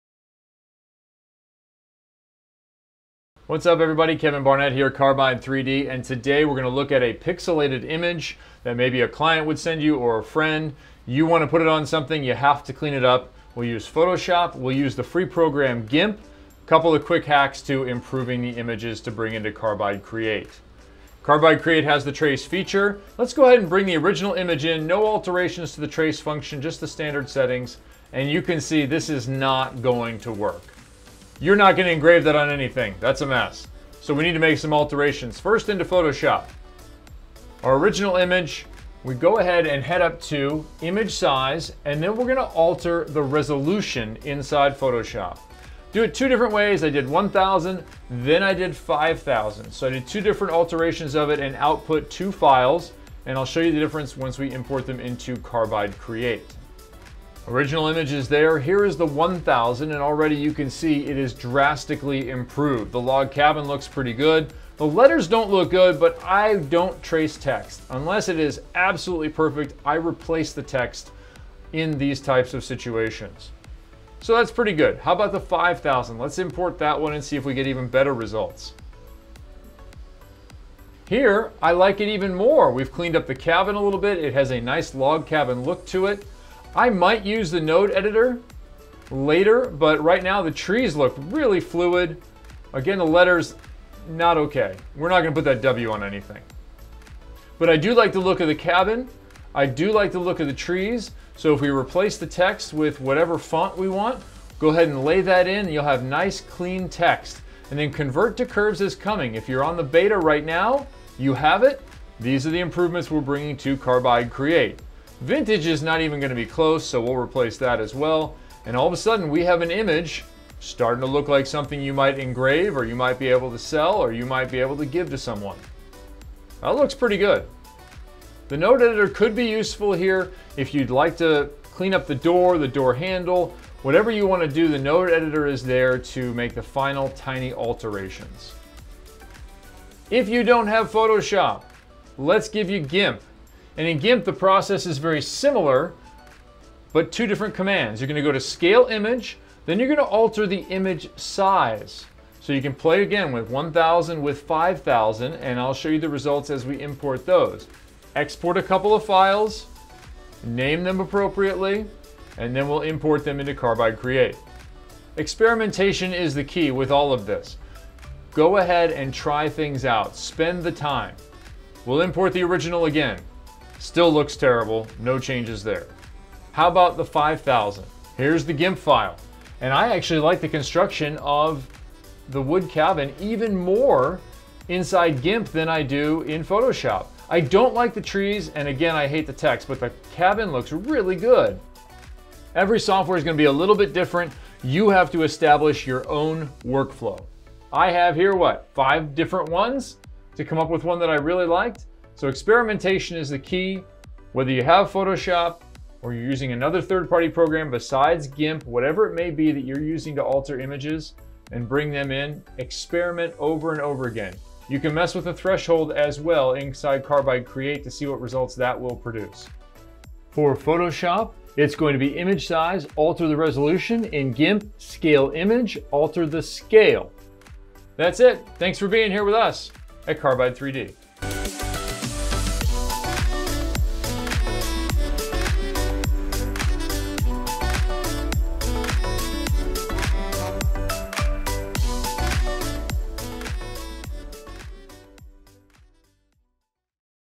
4 Minutes to Better Tracing of Pixelated Drawings
Have you struggled with taking a pixelated drawing and applying it in Carbide Create?   Here's a quick look at one strategy for creating a better Trace inside the software.   With either paid or free software you can improve the image quality and save yourself time in the project.